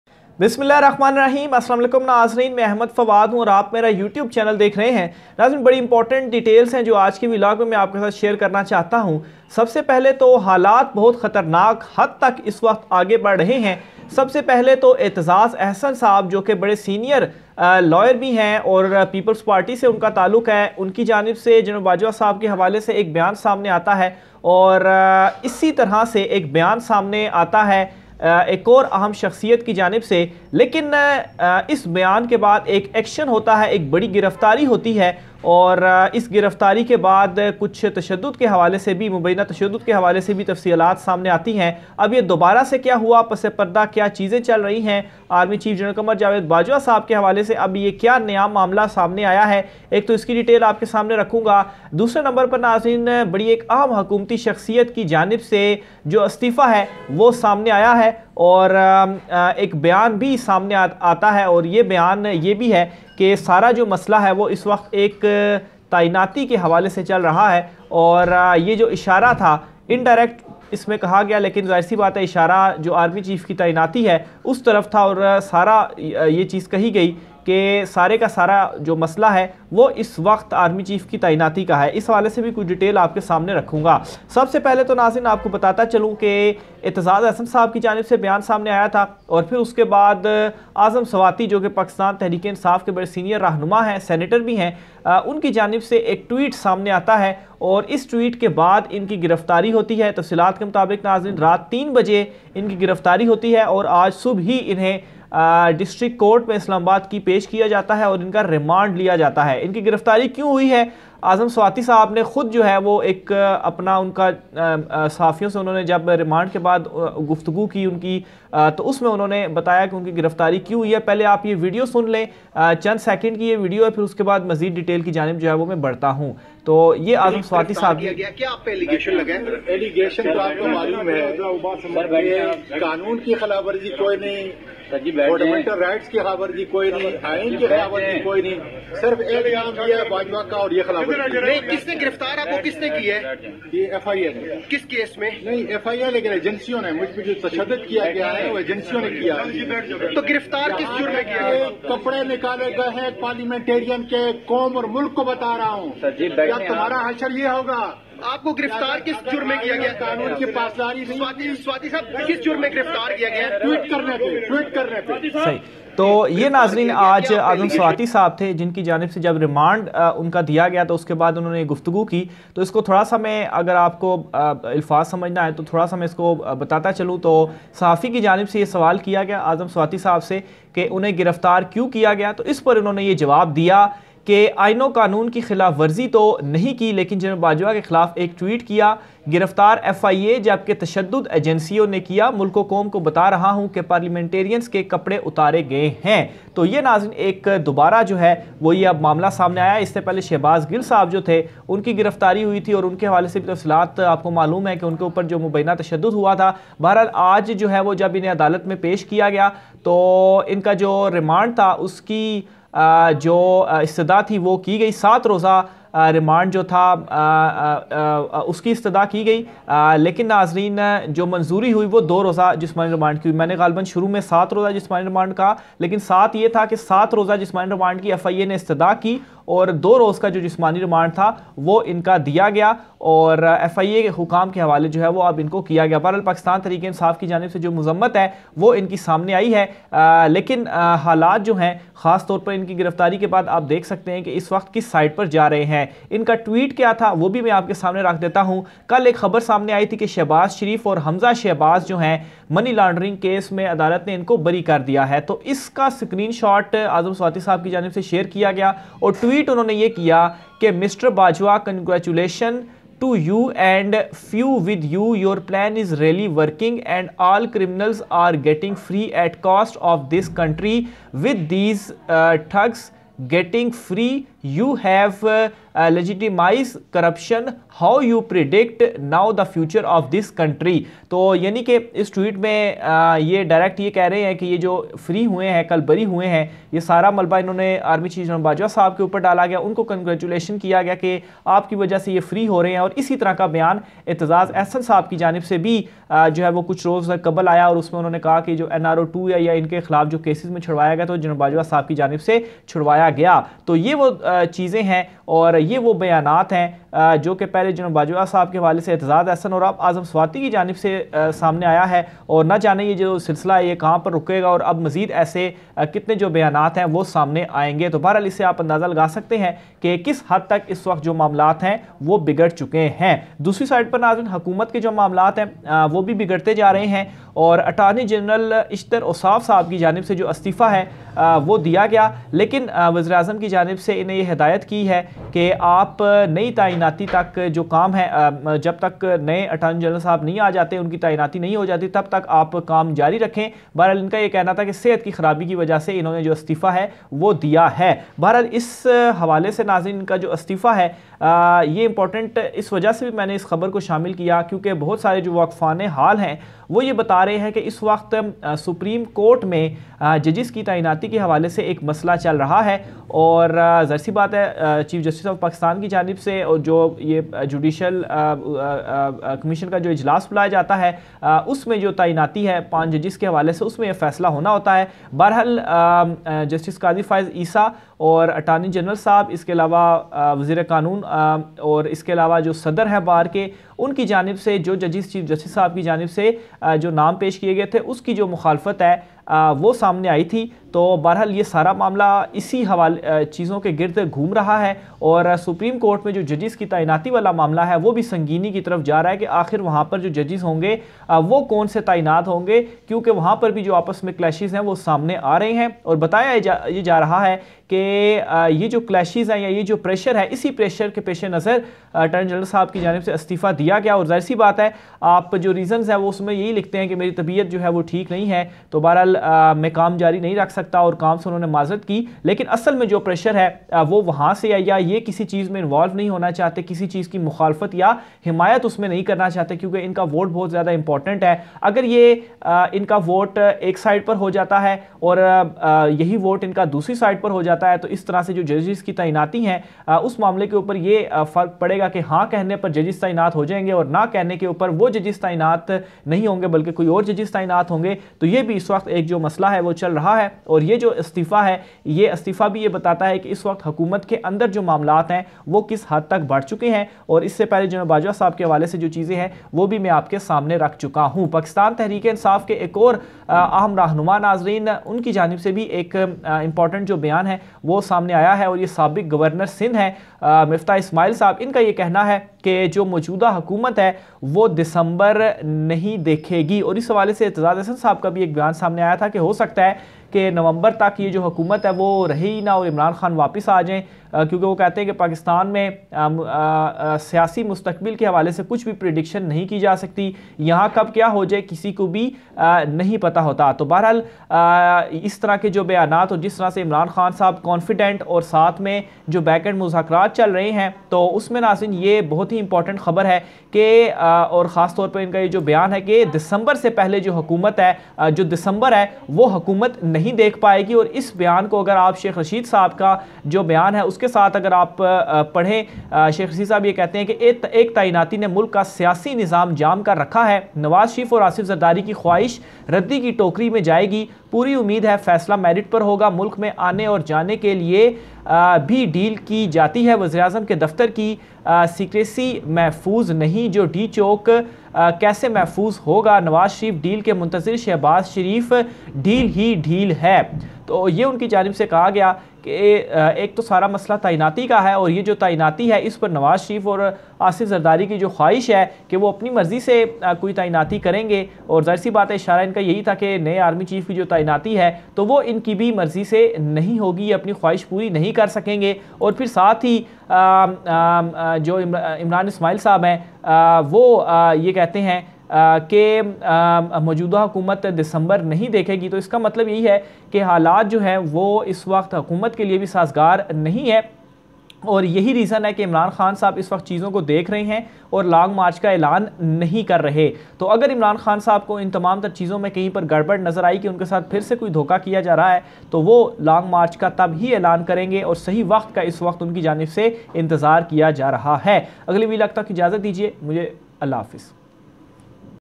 دسمبر سے پہلے شہباز حکومت کا خاتمہ || جنرل باجوہ نے کیا کر دیا؟ ویڈیو وائرل۔۔۔
details about azam sawati arrest?